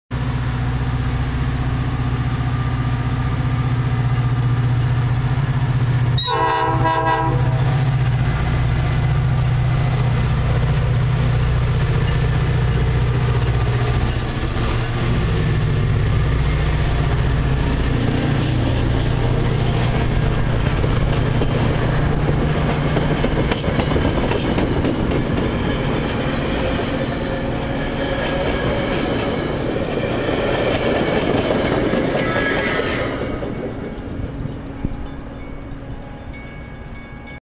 Metra 614 at Northbrook, IL
Metra 614 an EMD F40C loco leads MD-N train #2155 Westbound at Northbrook, IL on the CP C&M sub. Recorded on 12/9/2011